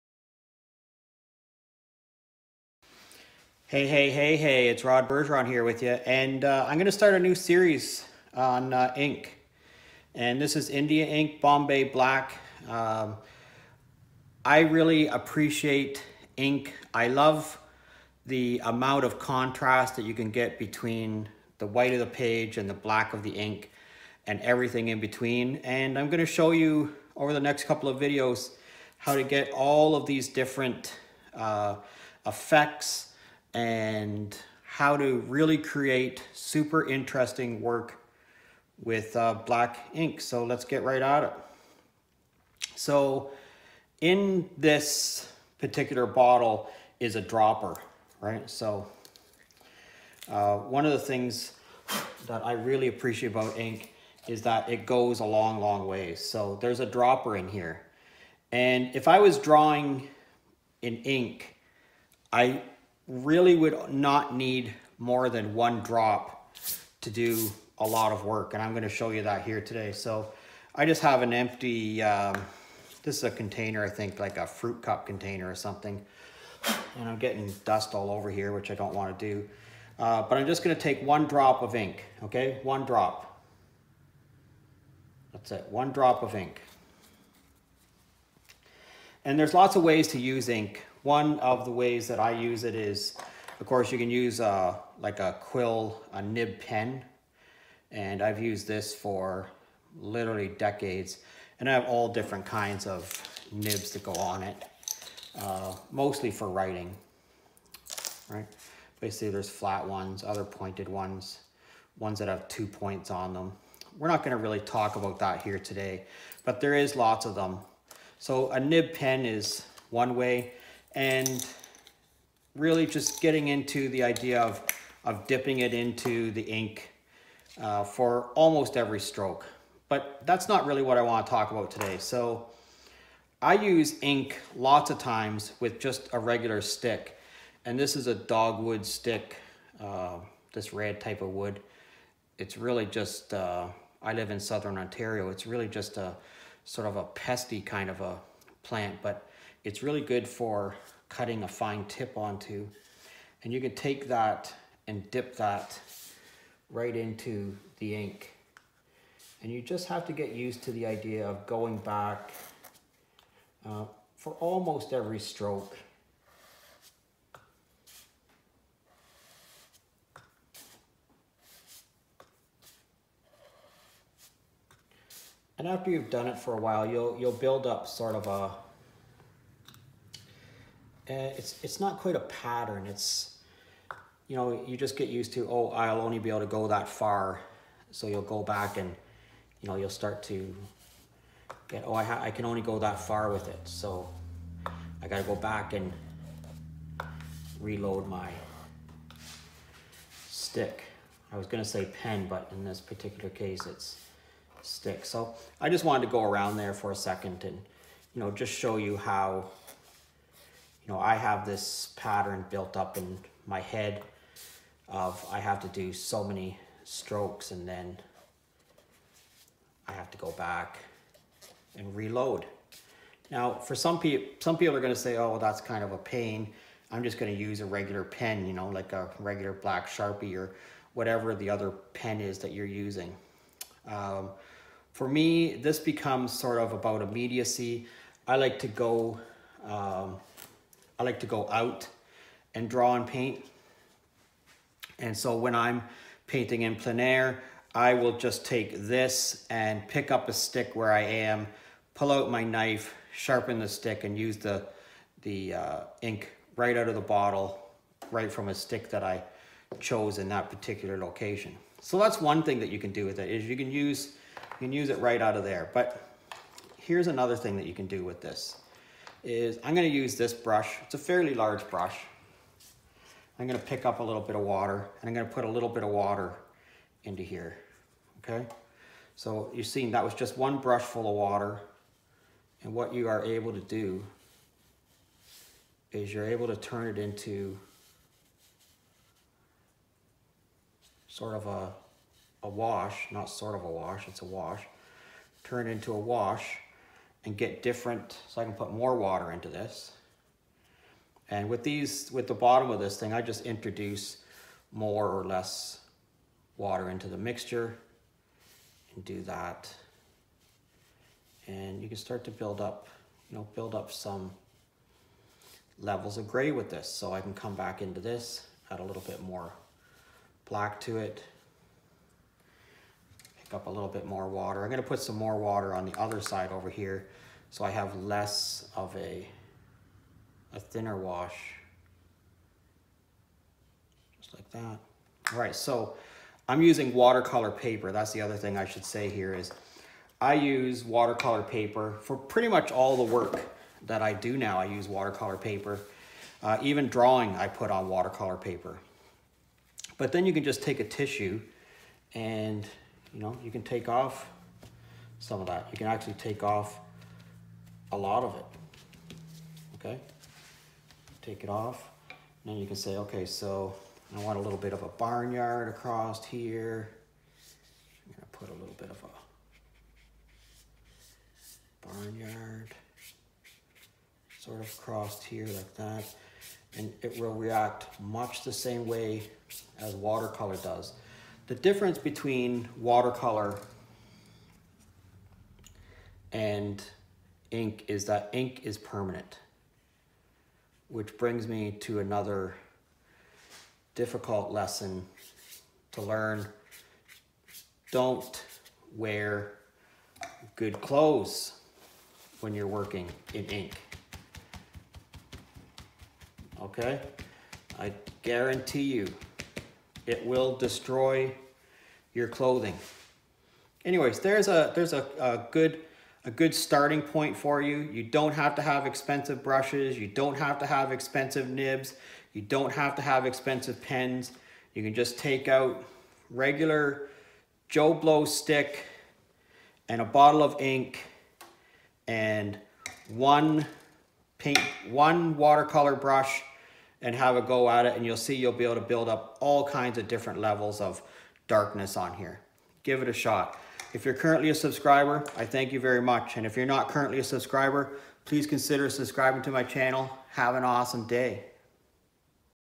PEN AND INK TUTORIAL, LINE AND WASH
Pen and ink is a cost effective way to create art. There are no special tools required and normal watercolour paper is best.

Links to stuff I actually use;
Weldbond Glue,
Sennelier Watercolour,
Moleskin 5X8.25 watercolour notebook,
Canson Watercolour Paper,
Sharpie Pens,